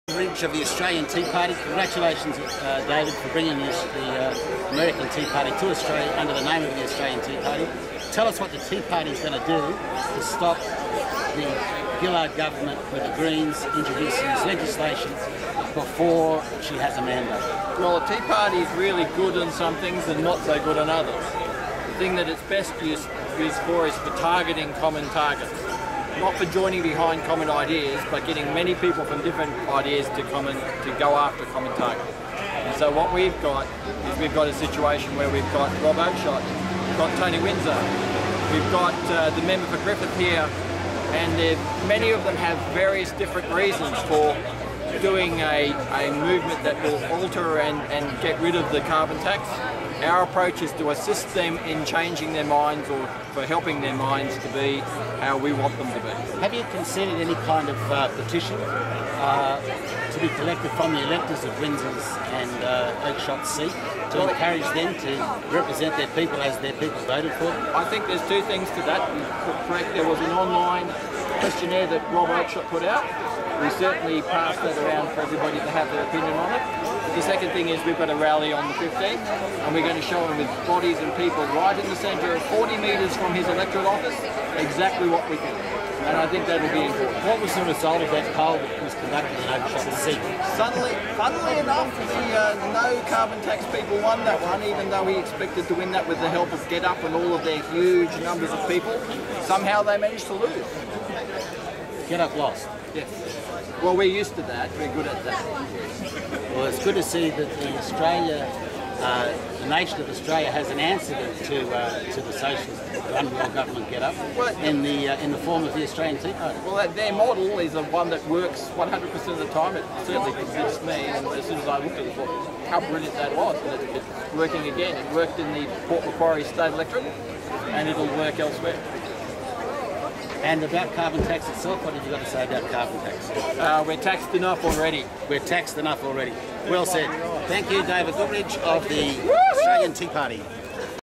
David Goodridge Aus TEA Party
7th of May, 2011 Anti Carbon Tax Rally